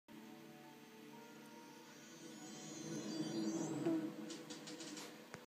Wally's dramatic fart
The music in the background sets the mood for the moment Wally does what he does best!